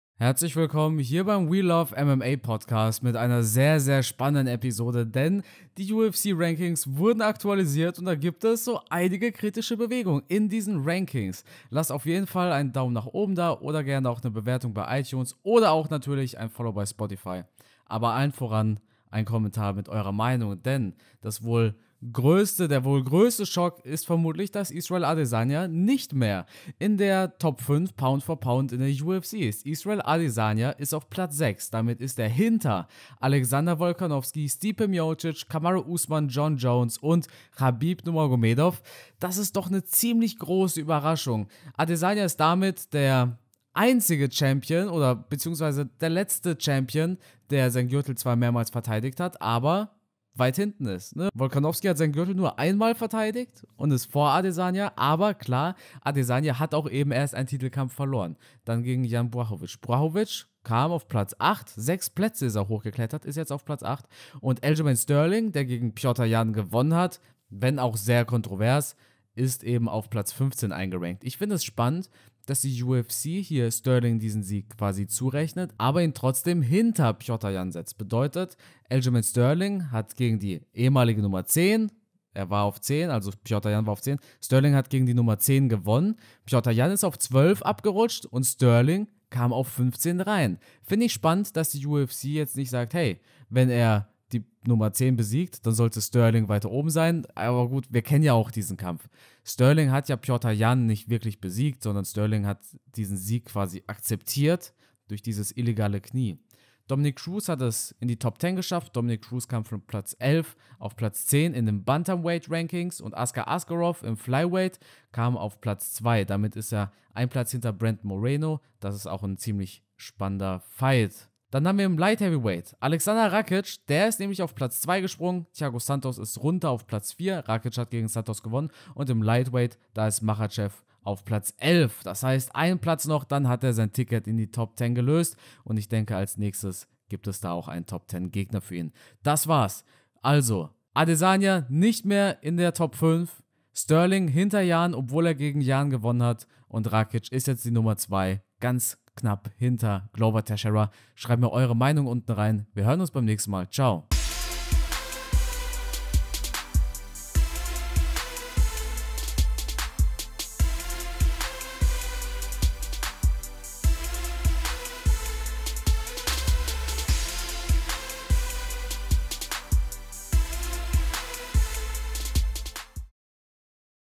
News: Israel Adesanya kein Top 5 P4P Kämpfer mehr!
Nach UFC 259 gab es einige spannende Bewegungen in den Rankings: Israel Adesanya ist nicht mehr in der Top 5 P4P, Aljamain Sterling schafft es in die Top 15 und mehr! Alle aktuellen Bewegungen gibt es in dieser Episode.